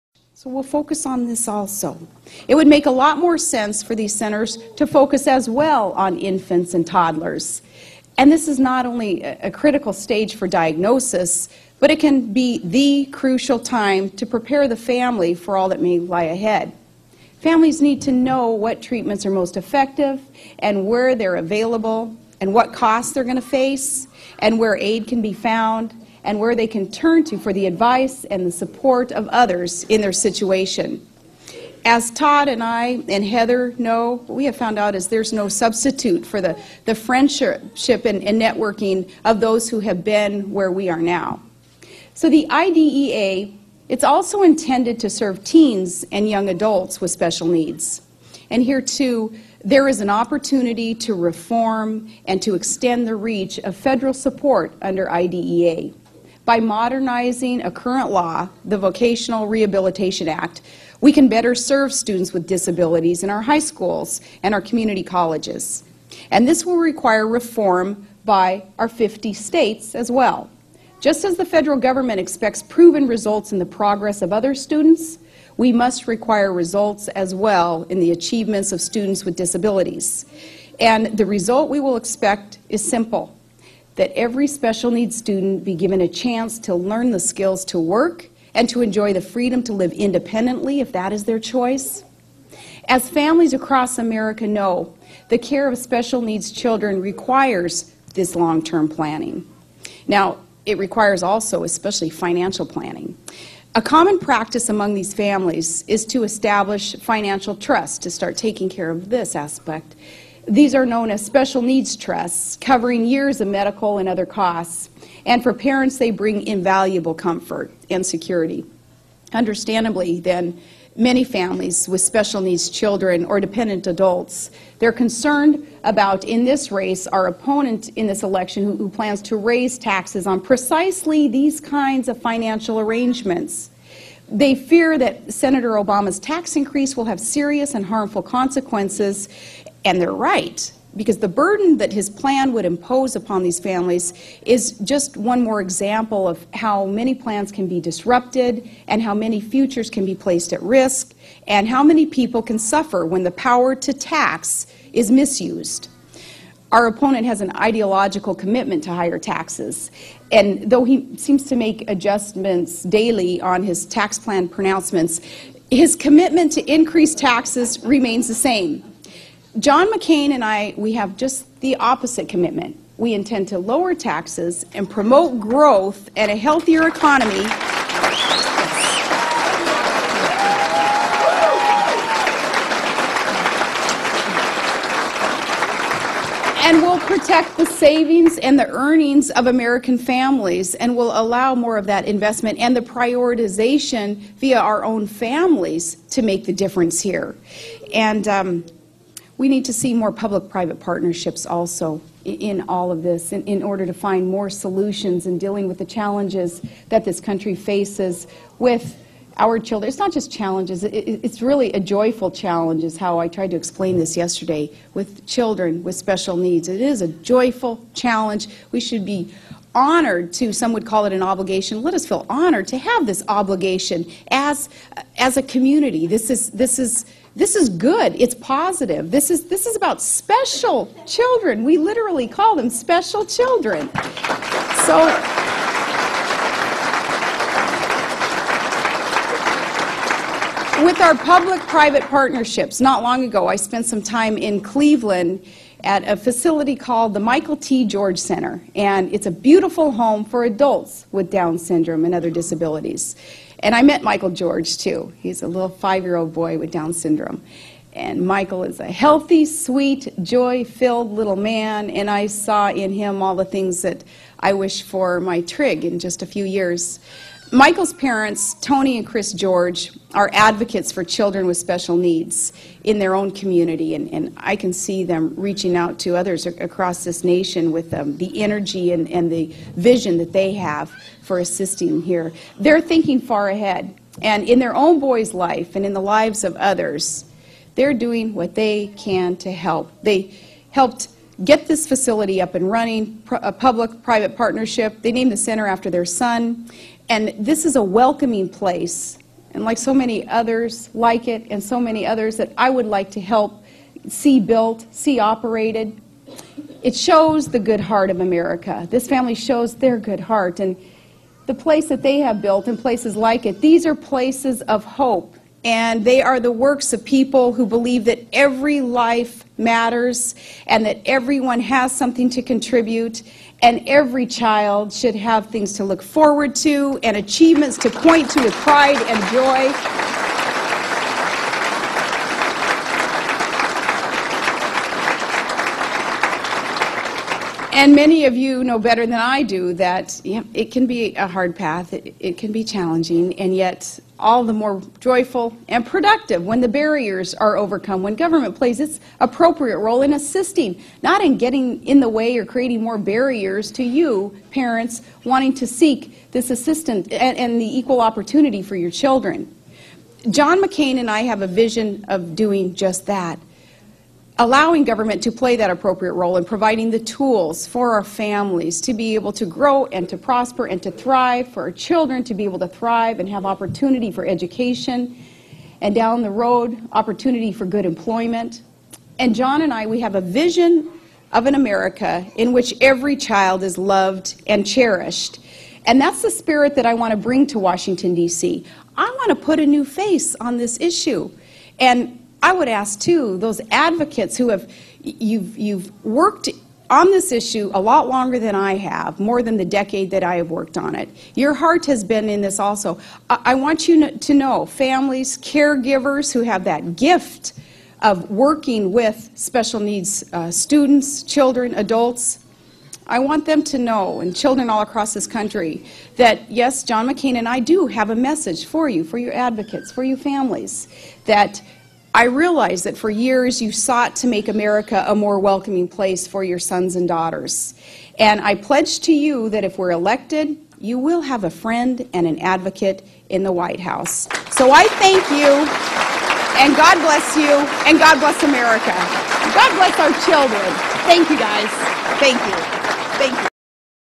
3 of 3 - Governor Palin's Speech on Children with Special Needs - October 24 2008
Governor Palin's Speech on Children with Special Needs - October 24 2008 - Part 3 of 3